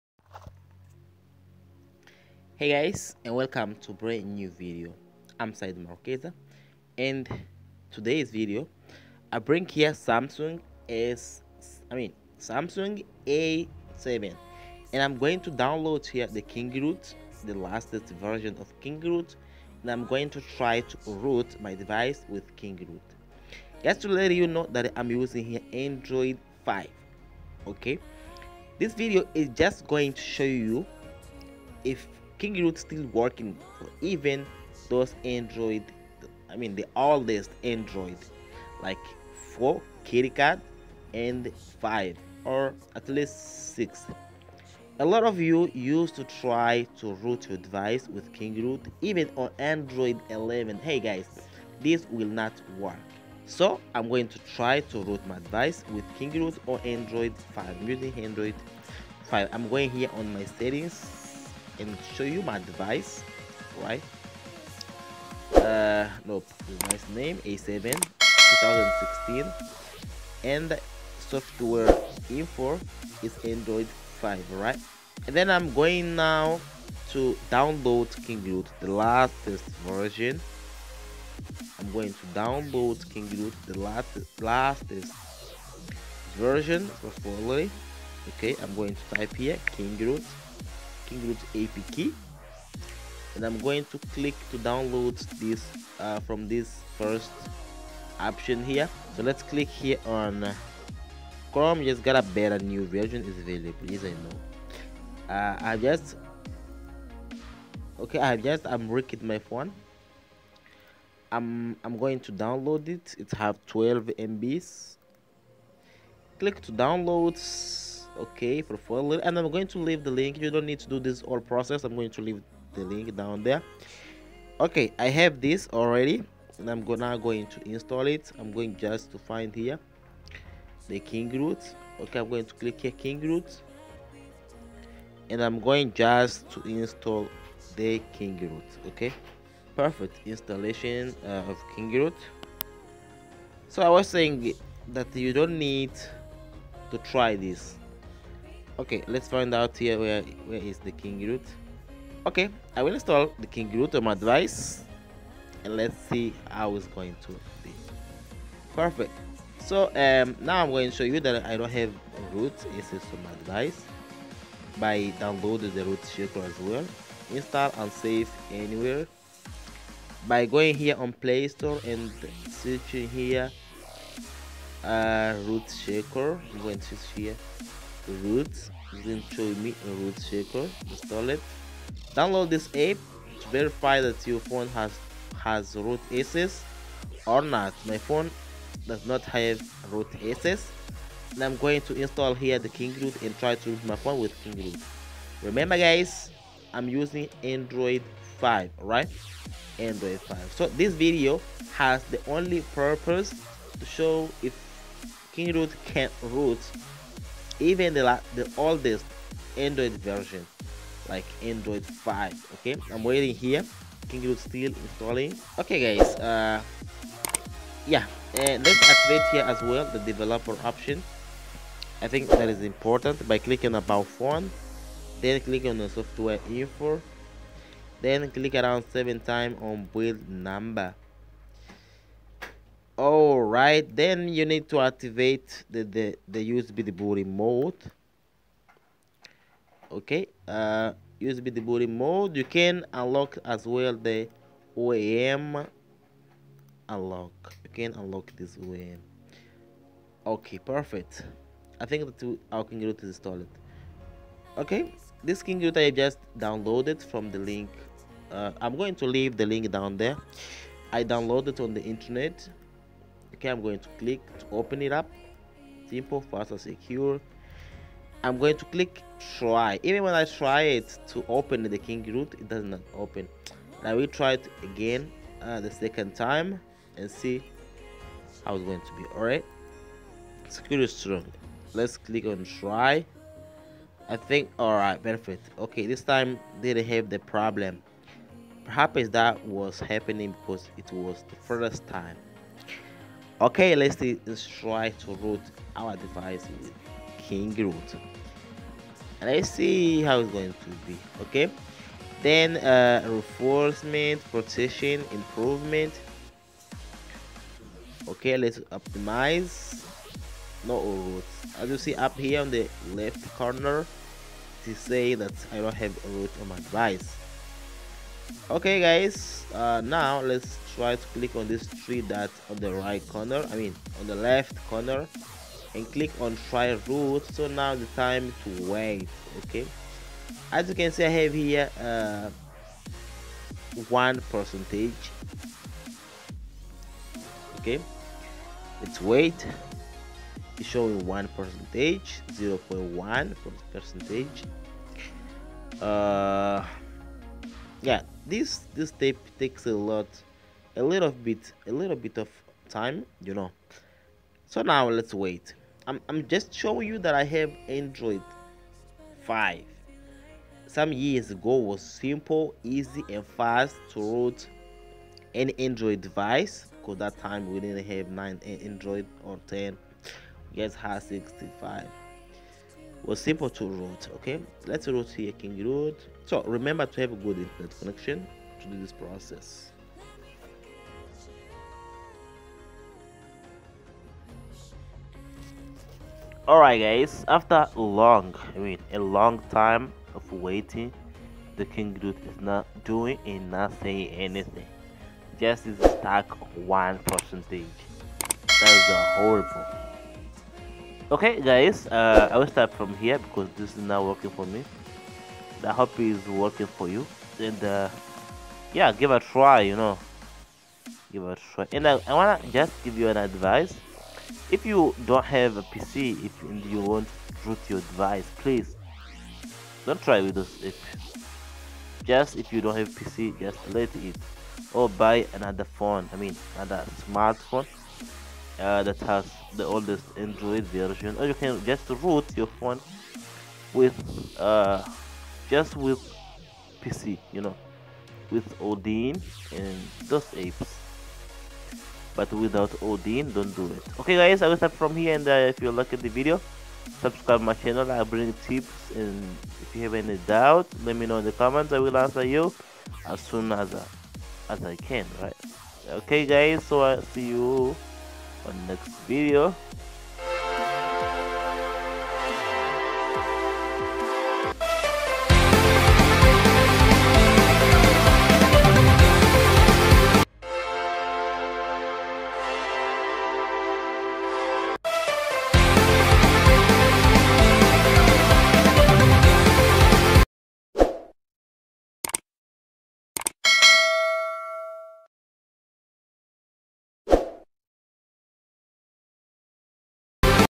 KINGROOT! Rooting Android Without PC | Watch THIS First Before Download
kingroot steal useful in  2021?
in this video I will show you by doing this process myself and we all understand on this video!

hOW tO ROOT ANY ANDROID DEVICE WITH KINGROOT

Thanks To the Vmos Team
NOTE: I did not create this, I did not modified this app I just downloaded already modified and I decide to share with you guys, so all the credits are reserved to the original owner of this mod/app
- This App/Rom is not for commercial use, it's just for entertainment only.